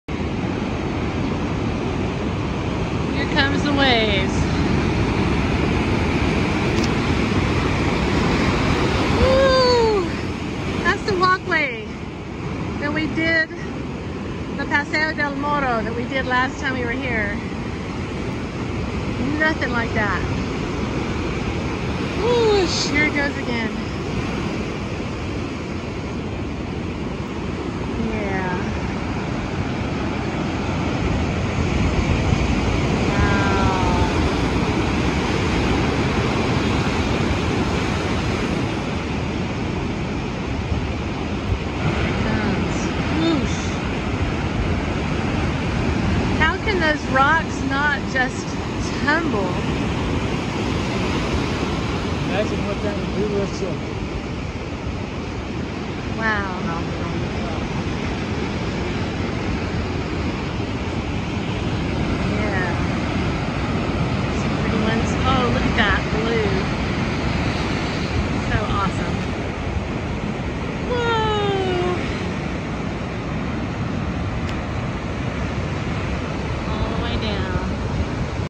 North Swell In Old San Juan
North swells from a low system in the Atlantic make for some dramatic waves crashing ashore in Old San Juan, PR